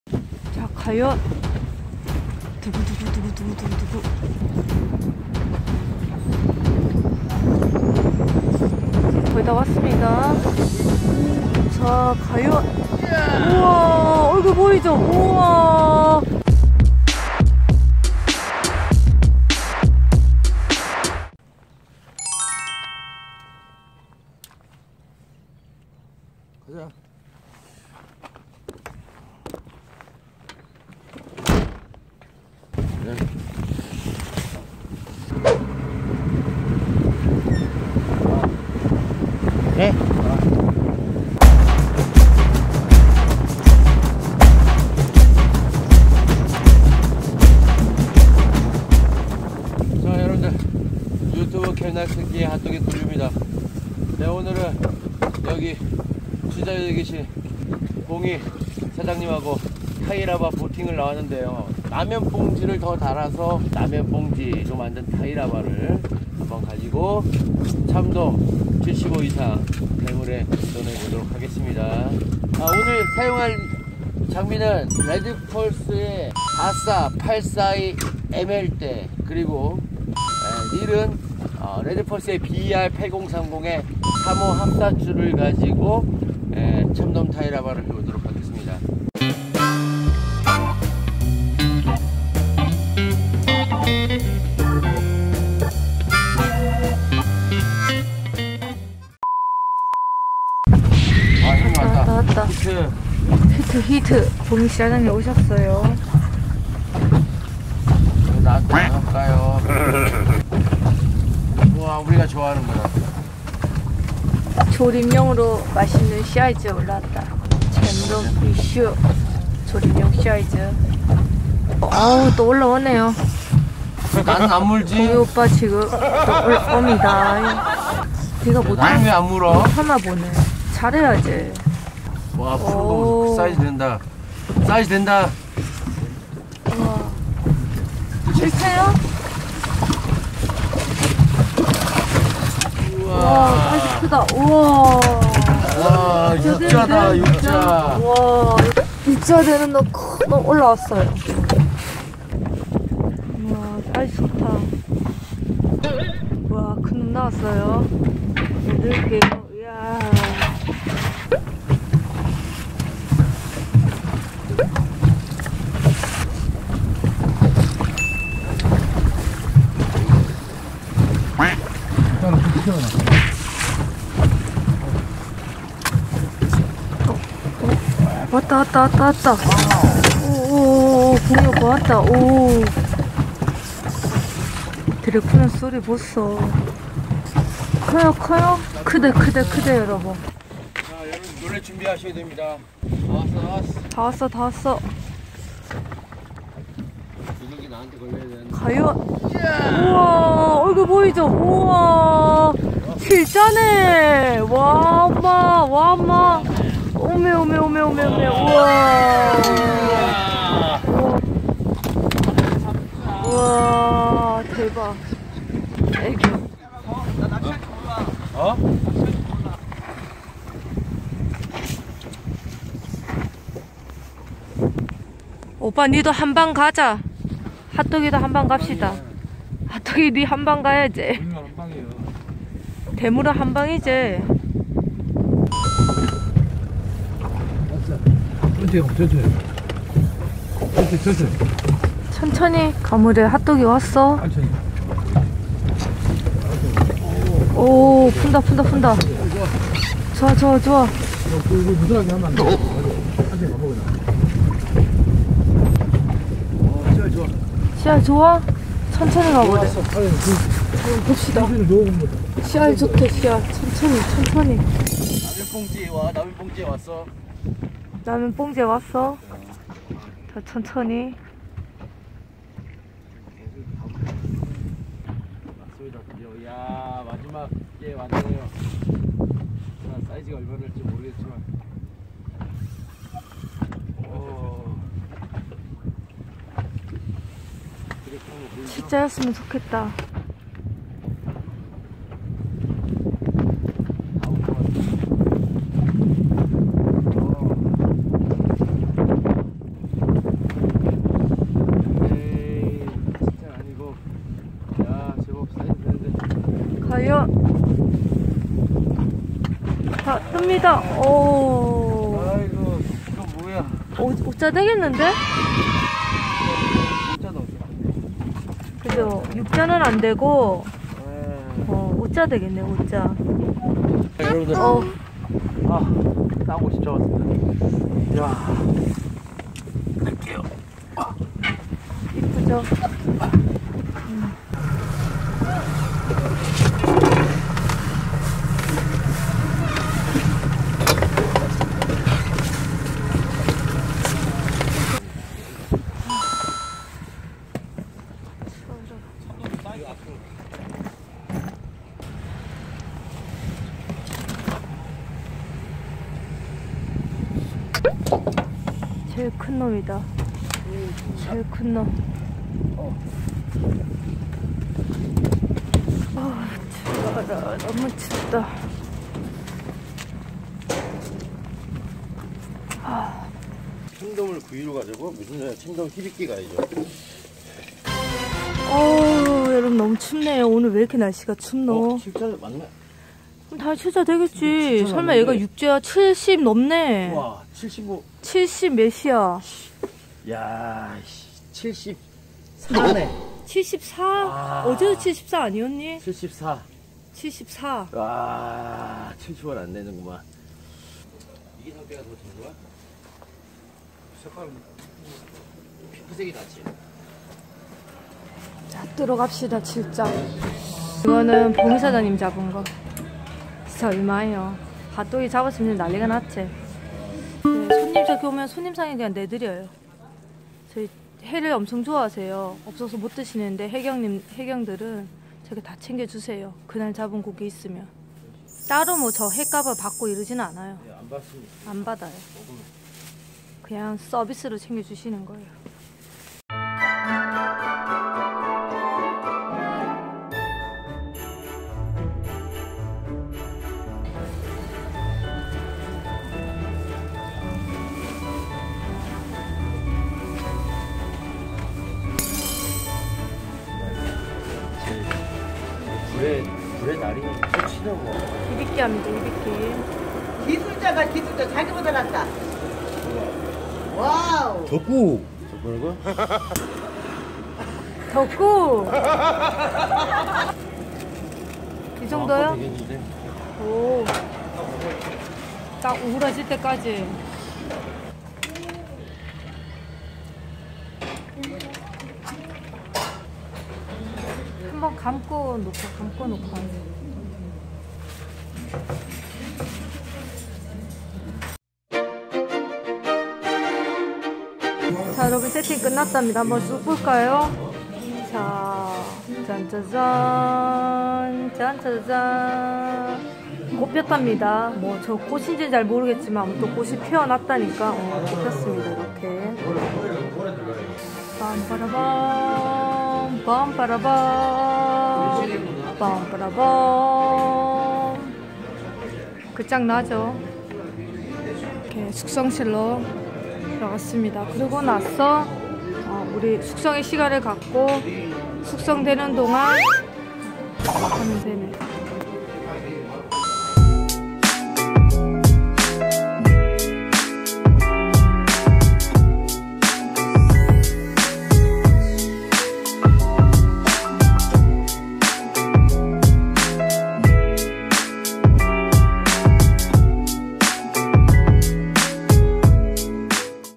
진라면봉지로 만든 타이라바! 참돔낚시 과연? [캠낚세끼] / 낚시n캠핑/ Camping&MukBang / Eating Show
[출조지:제주시 추자면 ]
[사용장비]
로드 : 레드펄스 다싸842ML
릴 : 레드펄스 BR8030
라인 : 합사3호
채비 : 타이라바